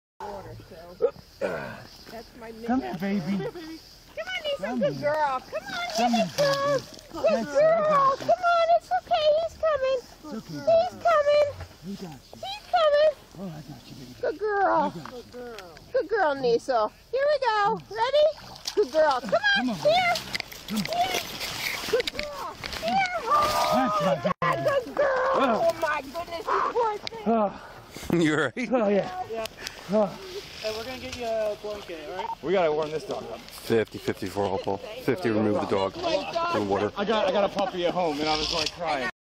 Officer Risks Life to Save Dog From Frozen Pond
Officers responded to a Pennington Harbourton Road property for the report of a dog that fell through the ice in a pond. The dog was unable to get out of the pond on its own.

Officers Robert Voorhees and George Peterson, along with Hopewell Valley Emergency Services and Pennington Fire Company personnel arrived at the scene to assist the dog’s owner.

Officer Voorhees volunteered to be fastened to a rope and crawled out on the edge of the ice towards the dog whose name is Nisel. Nisel swam towards Officer Voorhees who grabbed her by the collar. Officer Peterson and Specialist Dave VanDoren then pulled Officer Voorhees and Nisel to safety.

You will be directed to see more videos once you click the link!